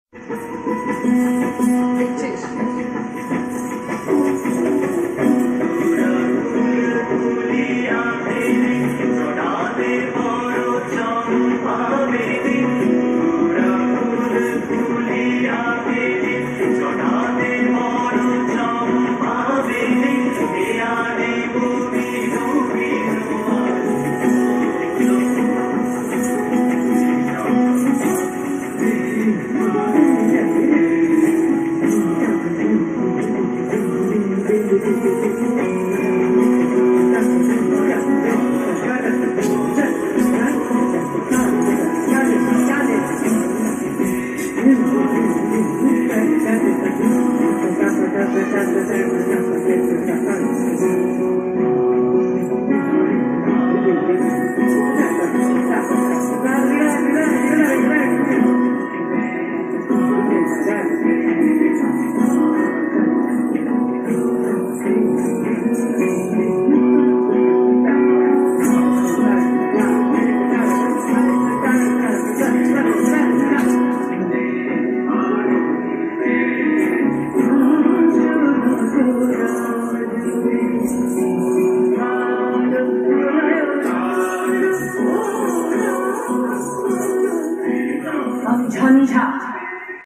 PUWAN (PUIPUI)IS TAKING DANCE TRAINING FROM HER MOM
VIDEO TAKEN BY ATASI MUKHERJI